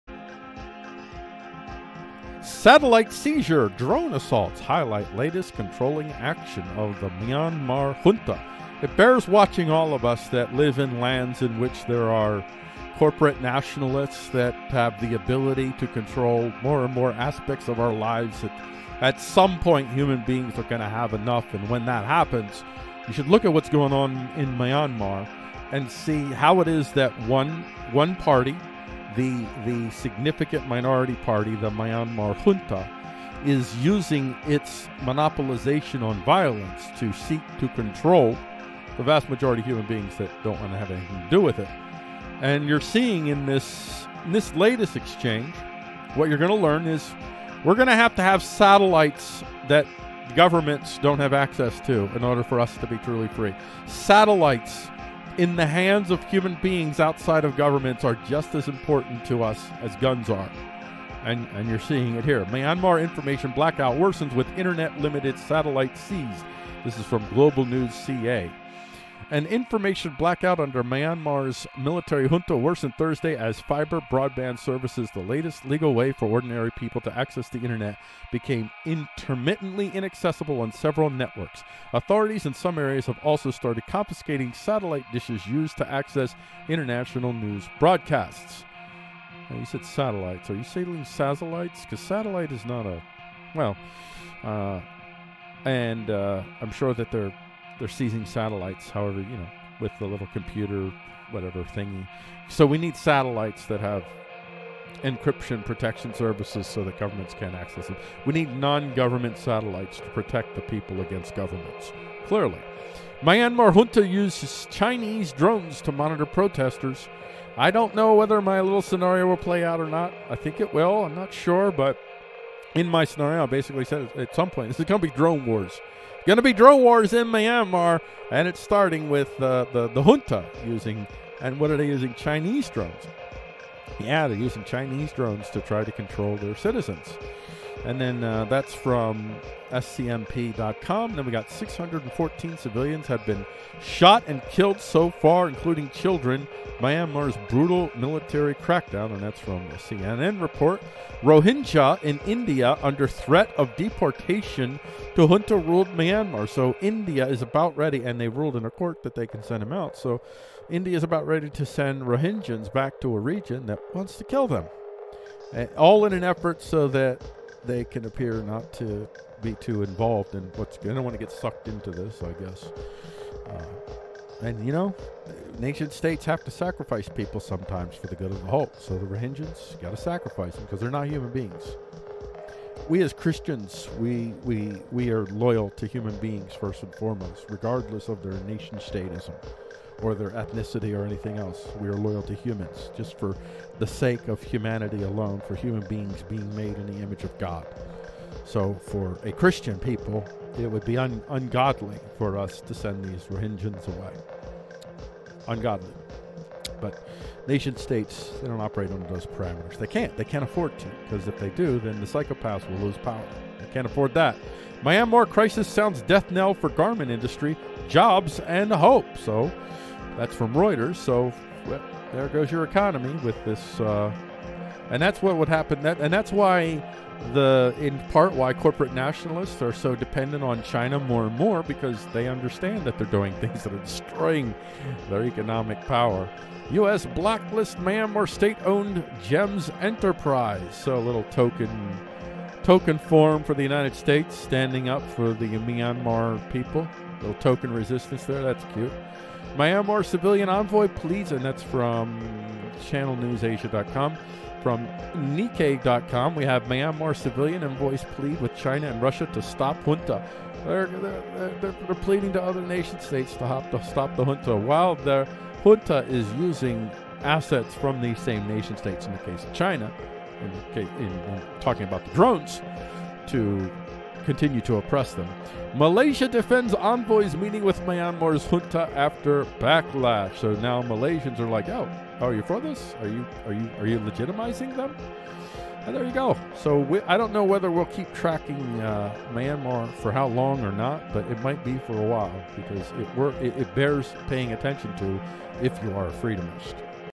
Satellite Seizure, Drone Assaults Highlight Latest Controlling Action of Myanmar Junta
Myanmar Crisis

Myanmar information blackout worsens with internet limited, satellites seized – National – globalnews.ca
From globalnews.ca Excerpt:   An information blackout under Myanmar’s military junta worsened Thursday as fiber broadband service, the last legal way for ordinary people to access the internet, became intermittently inaccessible on several networks. Authorities in some areas have also started confiscating satellite dishes used to access international news broadcasts.
Myanmar junta uses Chinese drones to monitor protesters: report – South China Morning Post
Excerpt: Myanmar junta uses Chinese drones to monitor protesters: report South China Morning Post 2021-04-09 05:00:04…
614 civilians had been shot and killed so far, including children – Myanmar’s brutal military crackdown CNN report
From /u/belkin626 Excerpt: submitted by /u/belkin626 [comments] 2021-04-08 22:09:50…